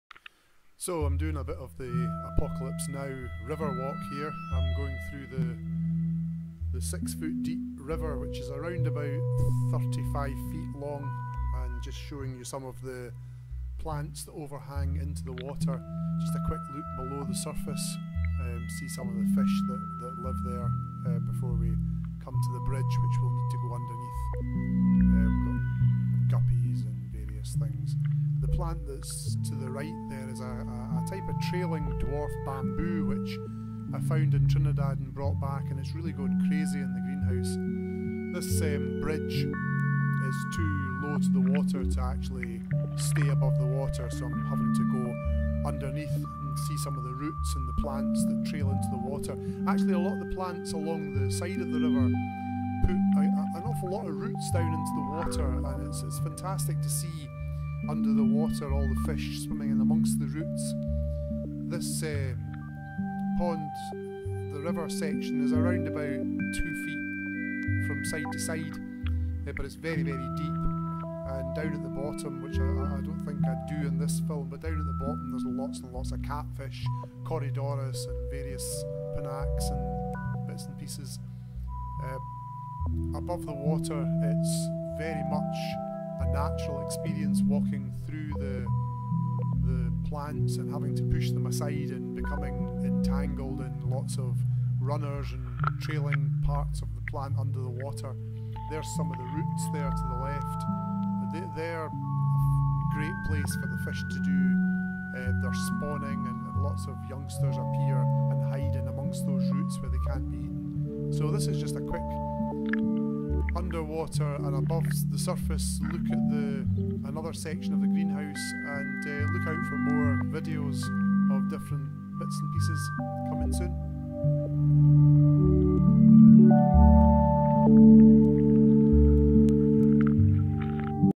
The river walk in Scottish Tropical Rainforest
Up to my neck in the 6ft deep river, surrounded by Sting Rays, Discus and a whole lot more, this is a quick film of how the greenhouse looks away from the paths and the easy to access areas. Its always an adventure because these areas are where the stuff that likes to hide away lives!